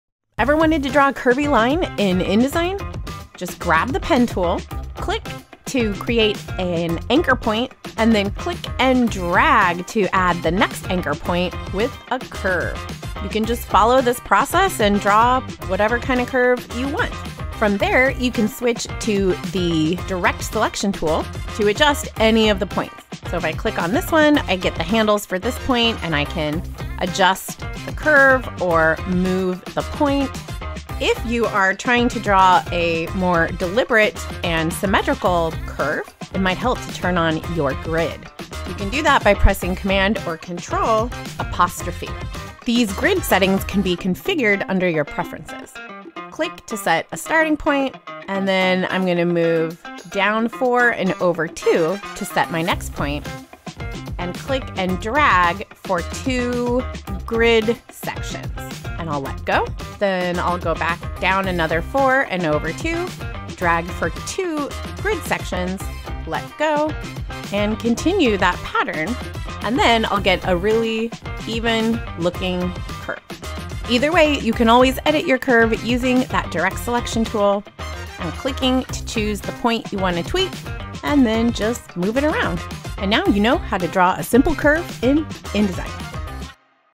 How to Draw a Curvy Line in InDesign
Khara Plicanic shows how to create a curved line in InDesign, using the Pen tool. She shows how to create points, adjust the curvature handles, and make adjustments with the Direct Selection tool. Lastly, Khara turns on the grid to easily create a symmetrical curved line.